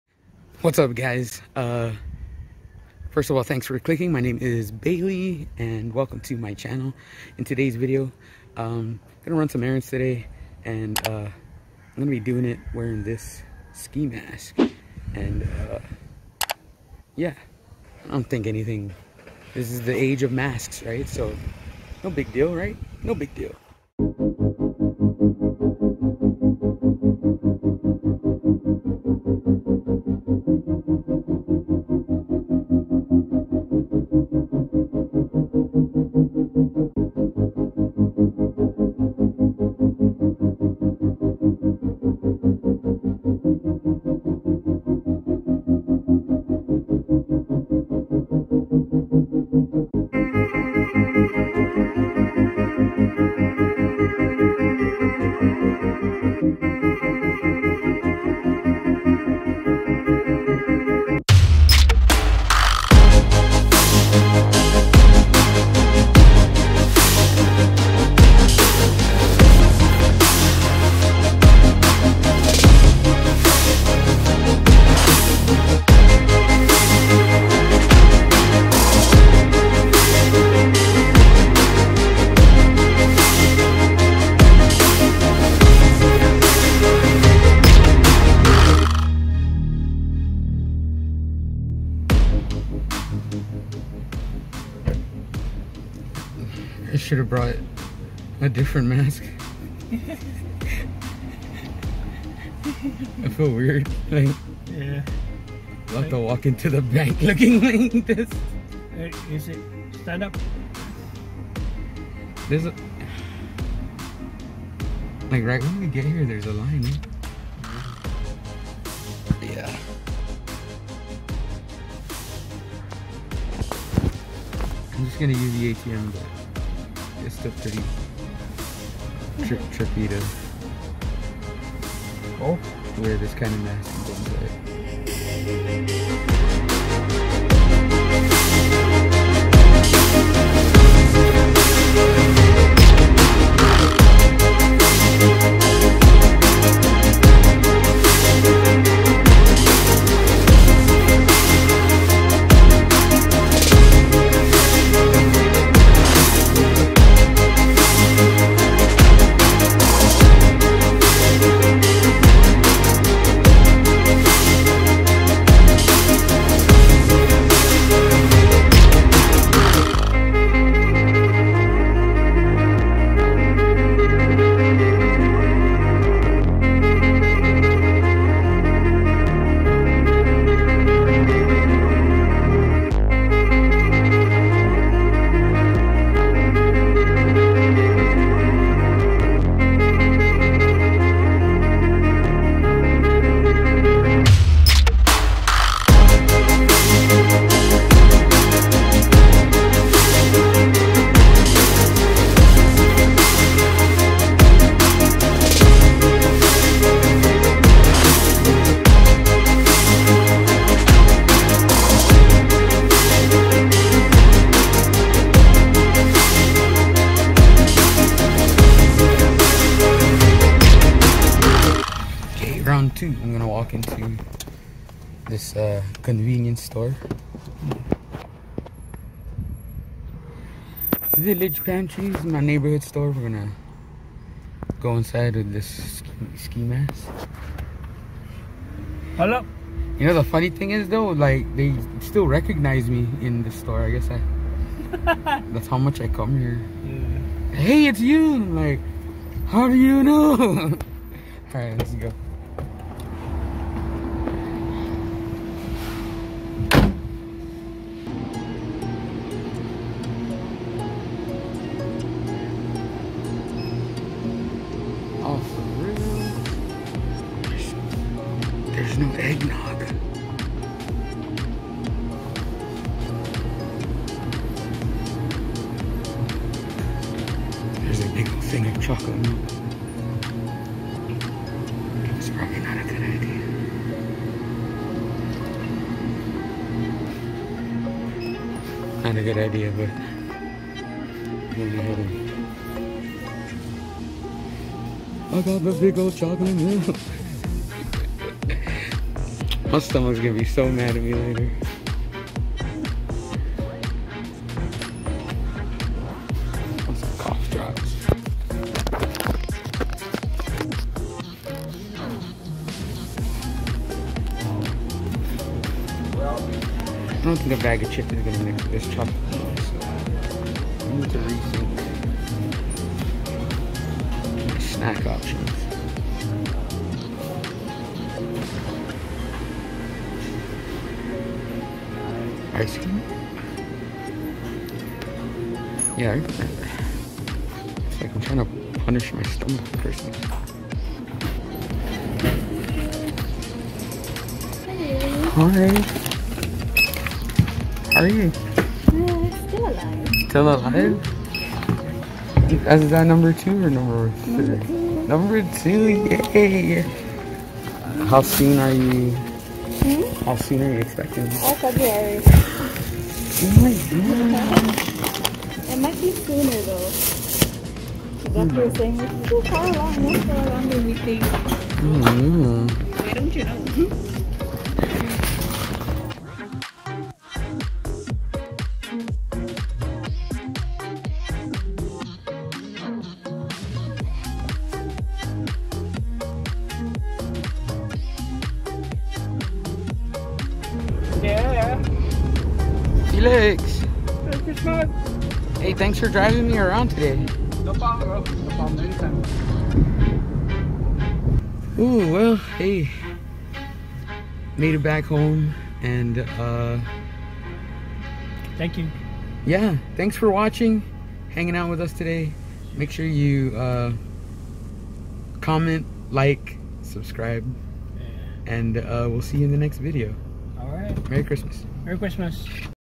Guam Life - The Age of Masks
what's up fam! today's video is a day in the life where I run some errands wearing a ski mask full face covering! we drive around the beautiful island of Guam and I deal with the anxiety and paranoia of looking like a criminal walking into a bank and a corner store.